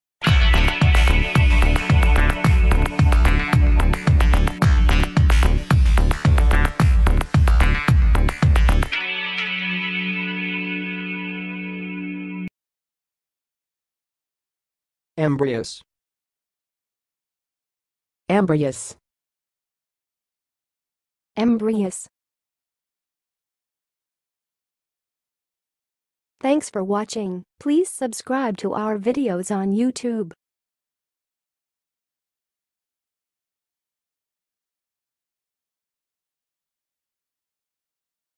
EMBRYOUS - How to pronounce Embryous?
EMBRYOUS -  Pronunciation, Meaning, and How to Use It in 2023

Embryos are the early and undeveloped stages of organisms, particularly in animals.
They are formed after fertilization and undergo growth and development until they reach maturity.
Embryos are characterized by rapid cell division and the formation of different body tissues and organs.
In humans, the term embryo specifically refers to the stage of development from fertilization until about the eighth week of gestation.

Pronunciation:
To pronounce 'embryos', you can say 'EM-bree-ohs' or 'EM-bree-oz'.

Test yourself by speaking the following examples:
1. The embryo undergoes rapid cell division before developing into a fetus
2. Embryos are the early stage of development in many animals
3. During gestation, the embryo forms different tissues and organs